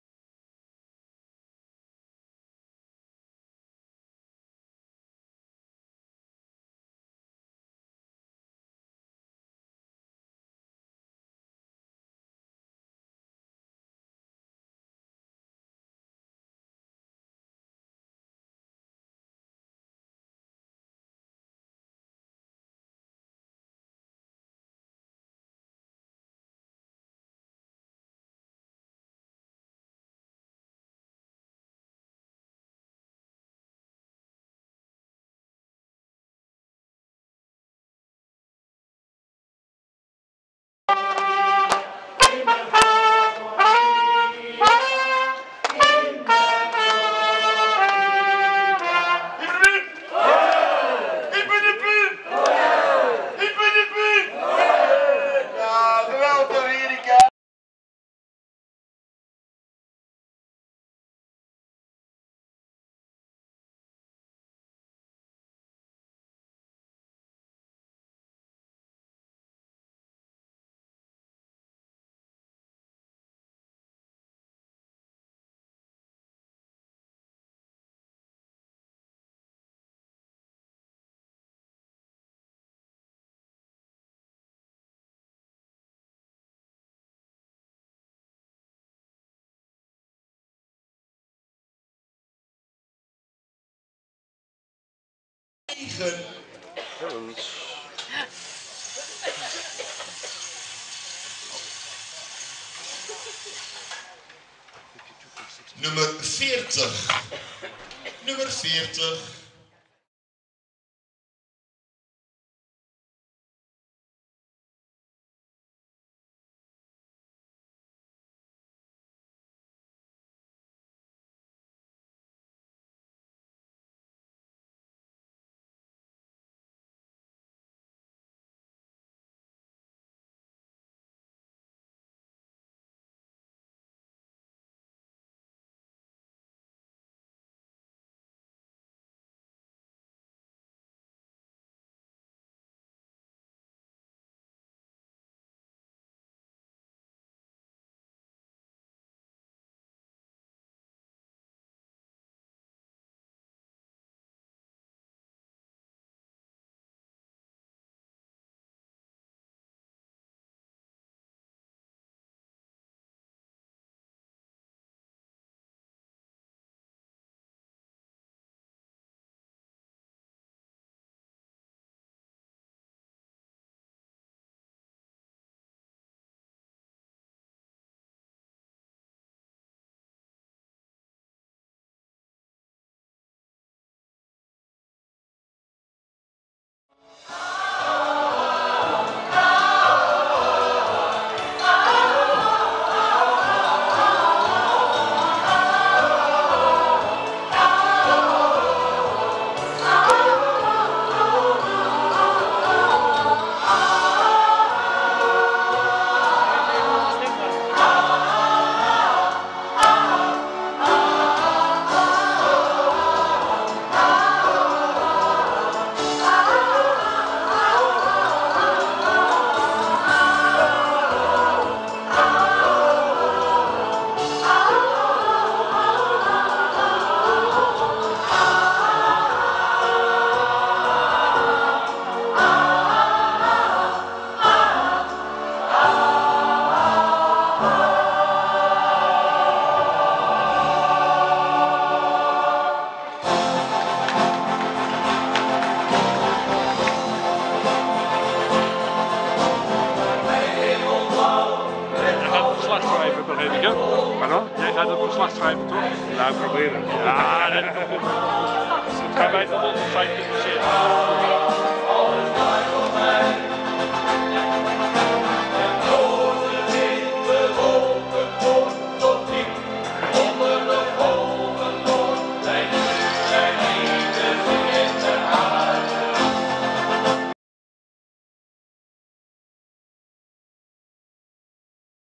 feest in de Veldspaat 22-02-2023
Een kleine compilatie van een feestelijke gebeuren in de zorginstelling Veldspaat in de mooie wijk Vinkhuizen, gesponsord door Univé